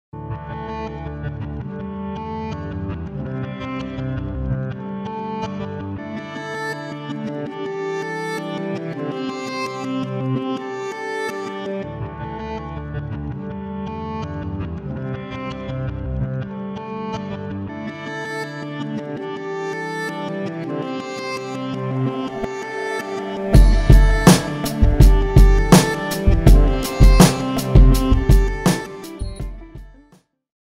Staffe ad aggancio rapido GTX per radiatori tubolari
Istruzioni di montaggio per radiatore: staffa a parete per aggancio rapido, con 2 chele trasparenti per il fissaggio del radiatore, adatta all'installazione di radiatori con altezza fino a 1.000 mm.